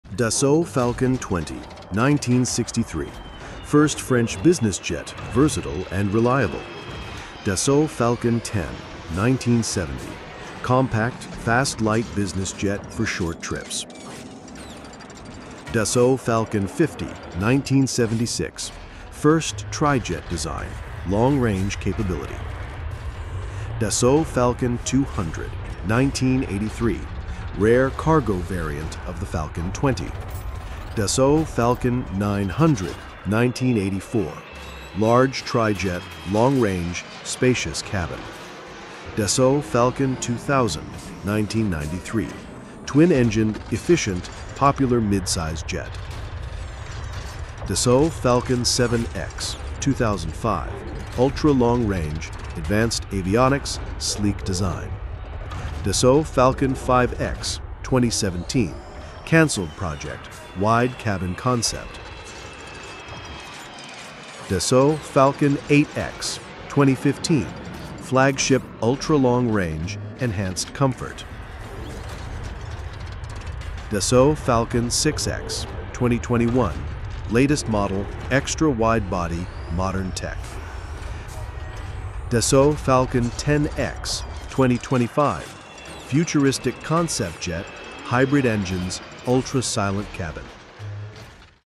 Evolution of French Luxury Jets 1963–2025 | Dassault Falcon Business Jets
Explore the full evolution of Dassault Falcon business jets from 1963 to 2025!
From the classic Falcon 20 to the futuristic Falcon 10X concept, see how French engineering shaped some of the world's most iconic private jets. Discover unique features, design milestones, and the vision for the future of luxury aviation.